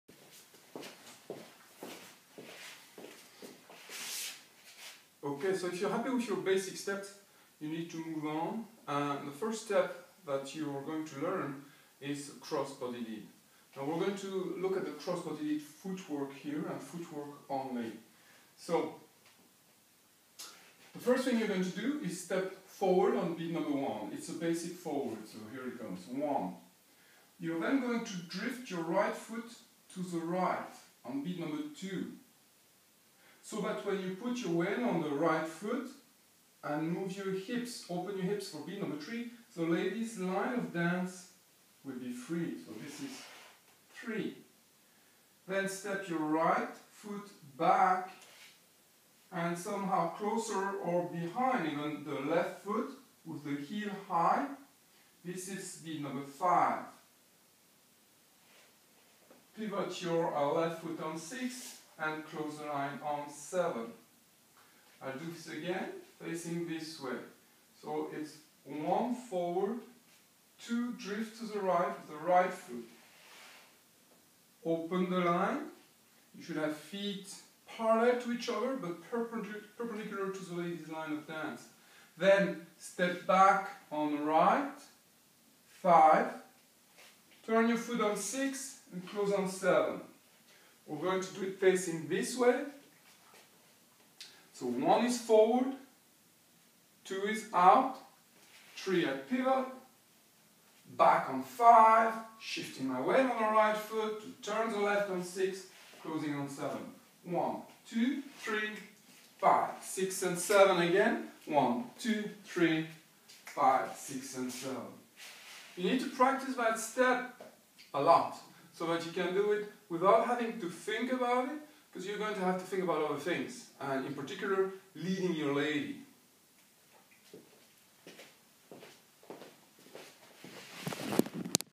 Salsa Nation - leader cross body lead footwork
Cross body lead footwork on 1 for the leader.
You need to practice this until you can do it in your sleep!
This footwork is used your all kind of cross body leads.